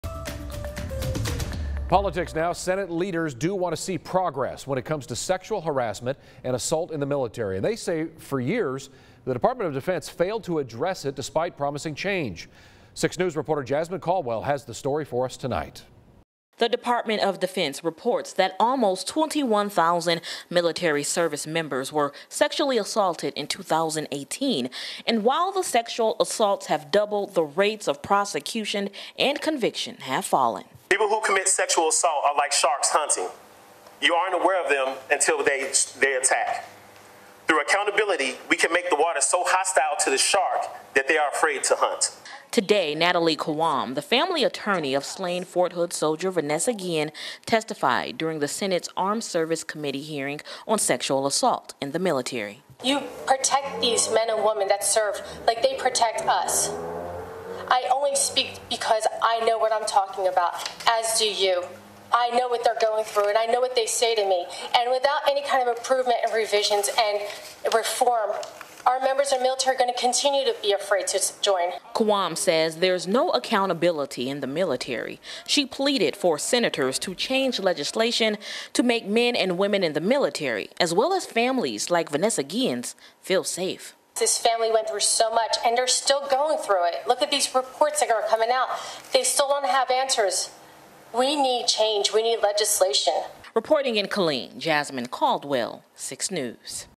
U.S. Senate committee investigates sexual assault in the military
Senators said for years that the Department of Defense failed to address sexual assault, despite past promises of change